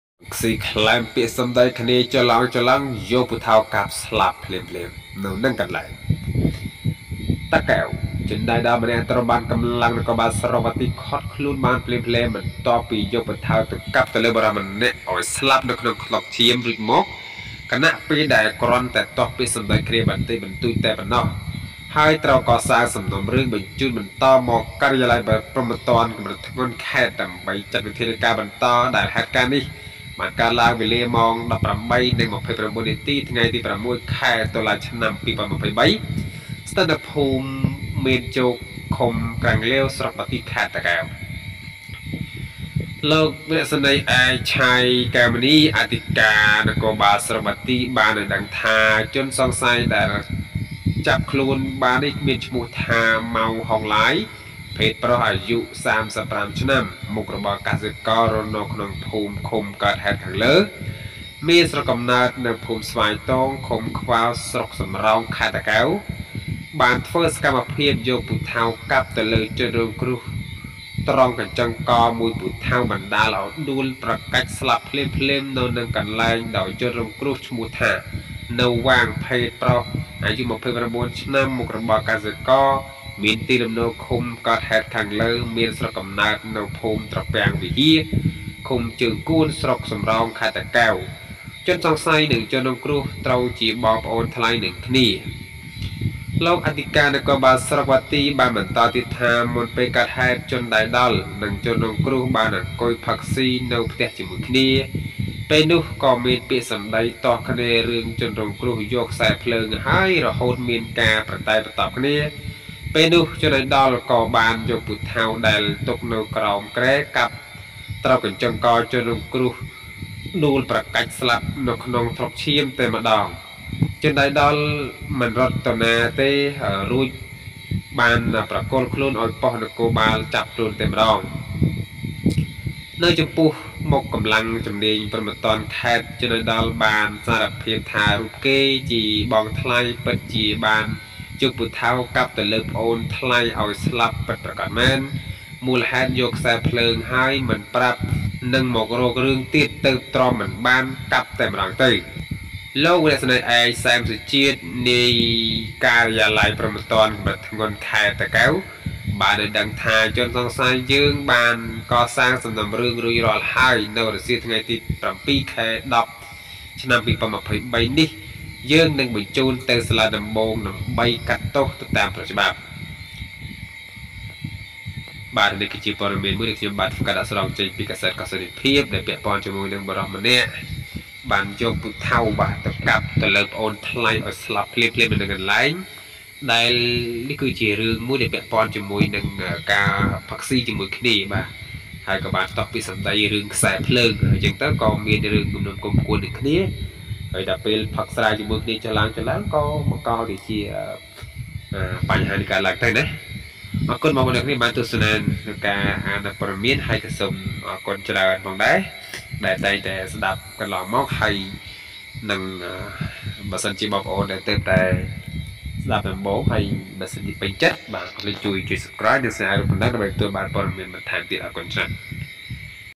ផឹកក្លែមៗតែពីរនាក់||កាប់គ្នាស្លាប់បាត់||ជម្លោះរវាងបងថ្លៃប្អូនថ្លៃ
ខេត្តតាកែវ ៖ ជន​ដៃដល់​ម្នាក់ ​ត្រូវ​​​បាន​កំលាំង​នគរបាល​ស្រុក​បាទី​ឃាត់ខ្លួន​បាន​ភ្លាម​ៗ ​បន្ទាប់​ពី​យក​ពូថៅ​កាប់​ទៅ​លើ​បុរស​ម្នាក់​អោយ​ស្លាប់​នៅ​ក្នុង​ថ្លុកឈាម​រួច​មក ខណ​:​ពេល​ដែល​គ្រាន់តែ​ទាស់​ពាក្យ​សំដី​គ្នា​បន្តិច​ប​ន្តួច​​តែ​ប៉ុណ្ណោះ​ ហើយ​ត្រូវ​កសាង​សំណុំរឿង​បញ្ជូន​បន្ត​មក​ការិយាល័យ​ព្រហ្ម​ទ​ណ្ទ​កំរិត​ធ្ងន់​ខេត្ត ​ដើម្បី​ចាត់ការ​បន្ត​ ដែលហេតុ​ការ​ណ៍​​នេះ​ បាន​កើតឡើង​វេលា​ម៉ោង​១៨ និង២៩​នាទី​ថ្ងៃ​ទី​០៦ ខែតុលា ឆ្នាំ​២០២៣ ស្ថិត​នៅ​ភូមិ​មានជោគ ឃុំ​ក្រាំង​លាវ ស្រុក​បាទី ខេត្តតា​កែវ​ ។